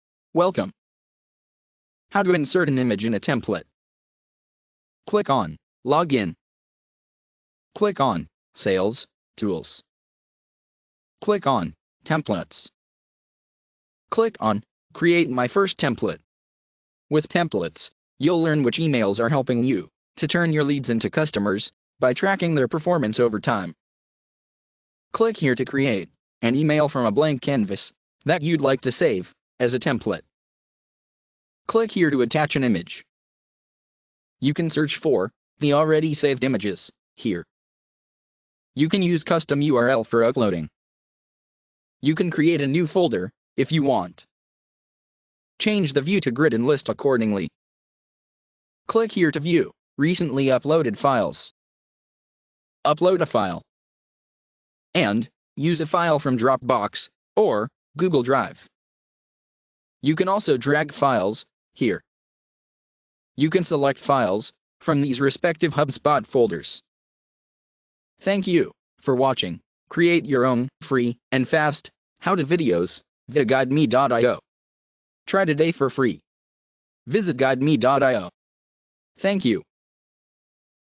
How to insert an image in a template
Tour : How to insert an image in a template

1. Click on Log in

2. Click on Sales Tools

3. Click on Templates

4. Click on create my first template
With Templates you’ll learn which emails are helping you to turn your leads into customers by tracking their performance over time

5. Click here to create an email from a blank canvas that you’d like to save as a template

6. Click here to attach an image!

7. You can search for the already saved images here

8. You can use custom URL for uploading

9. You can create a new folder if you want

10. Change the view to grid and list accordingly

11. Click here to view recently uploaded files

12. Upload a file

13. And, use a file from Dropbox or Google Drive

14. You can also drag files here

15. You can select files from these respective HubSpot folders